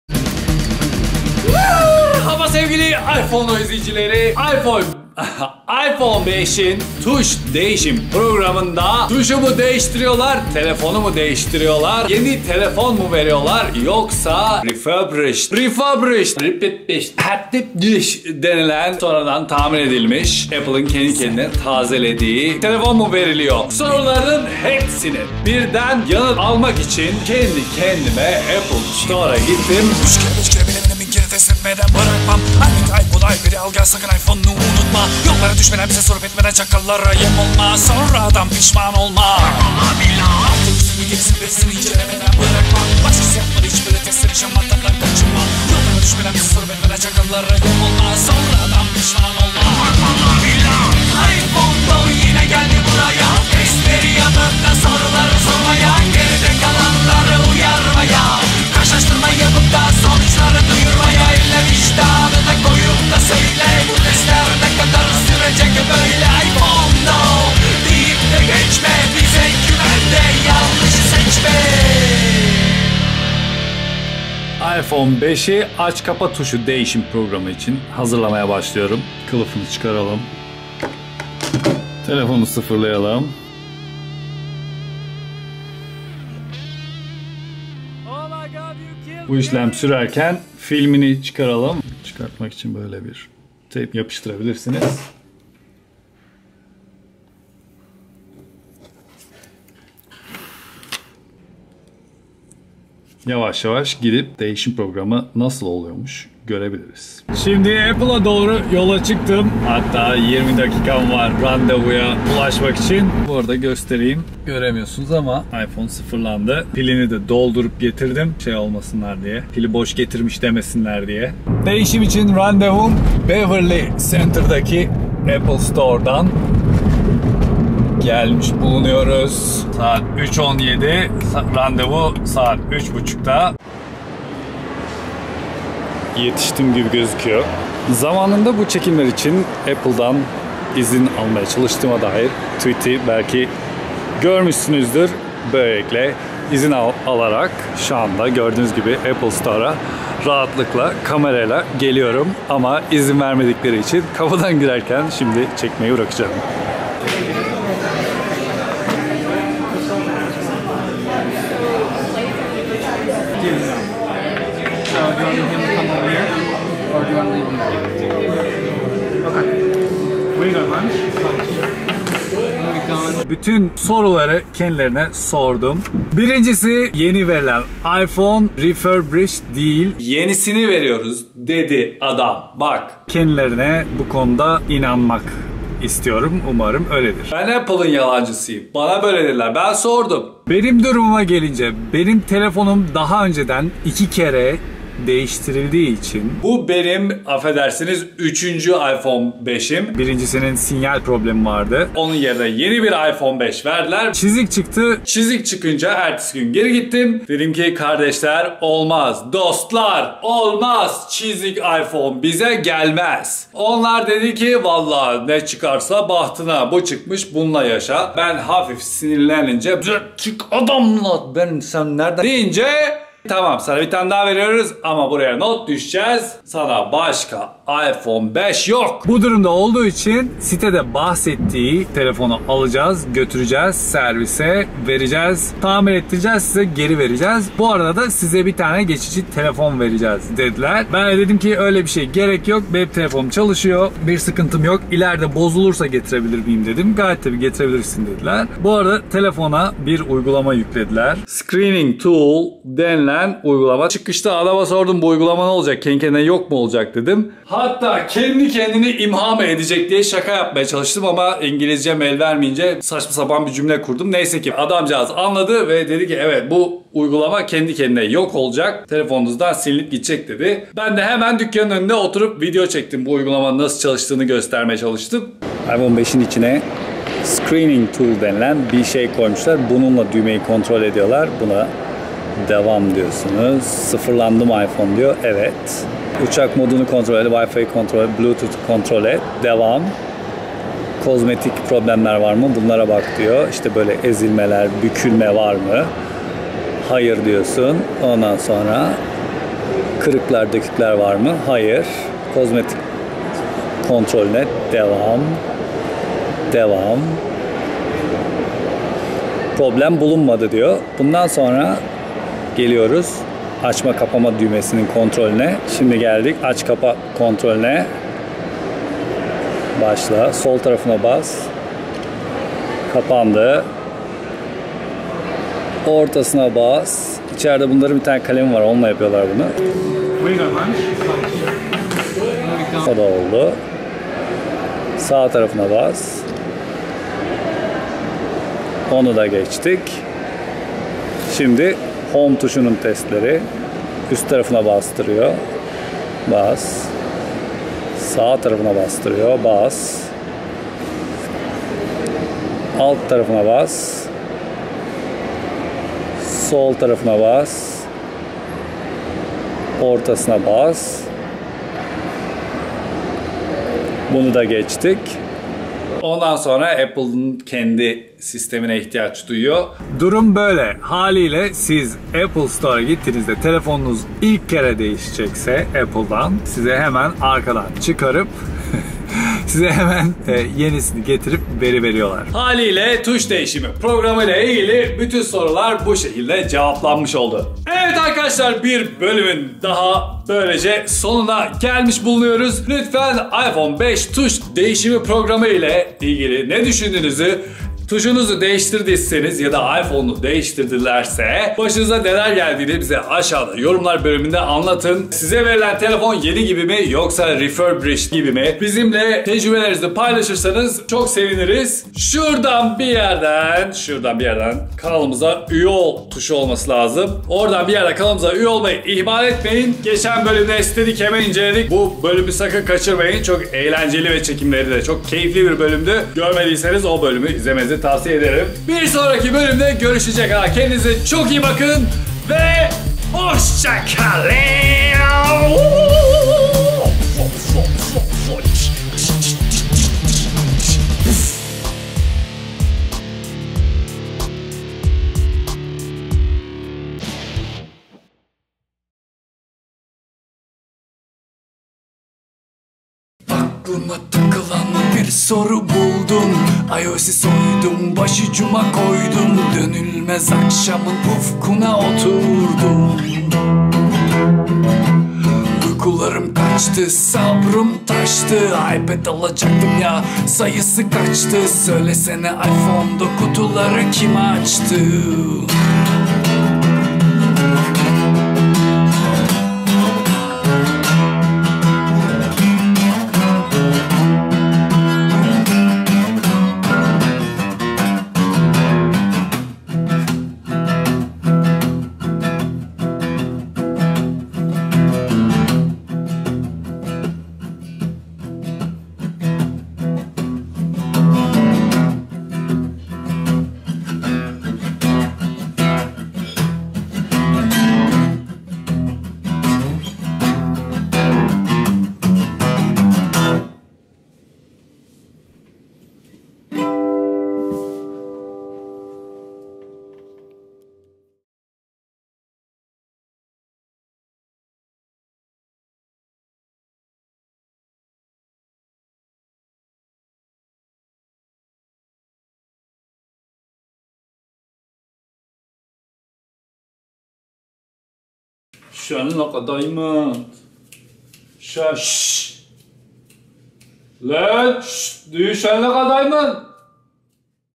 iPhone 5 Uyku/Uyanma Düğmesi Değiştirme Programı (CC in English)
Kimi iPhone 5'lerin aç/kapat tuşunda arıza olma ihtimaline karşı Apple değişim imkanı sunuyor. Peki bu değişim nasıl oluyor diye bir çok soru vardı, işte cevapları.

Ayrıca başka hiç bir yerde göremeyeceğiniz Screening Tool uygulaması incelemesi!

Apple'ın iPhone 5 Uyku/Uyanma Düğme Değiştirme Programı hakkında daha fazla bilgi

ENGLISH: I went to the Apple Store and got a lot of information about iPhone 5 Sleep/Wake Button Replacement Program. I also got to see an app called Screening Tool that they use. Which stops working after a while.

PLEASE TURN ON CC FOR SUBTITLES IN ENGLISH